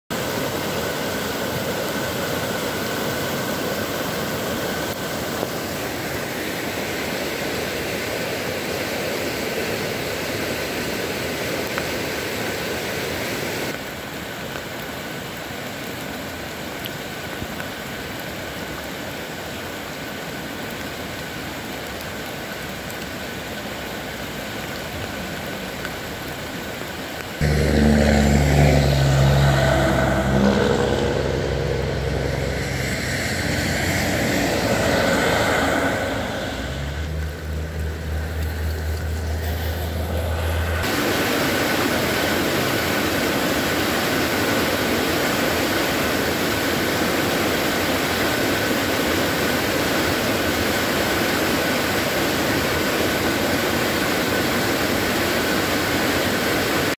Local Flooding in Gorst WA as the Atmospheric River Event Begins
Taken 2/28 on Belfair Valley Rd in Gorst. This is the start of this event which would give the area over 7' of rain in a 48 hour period by 3/1.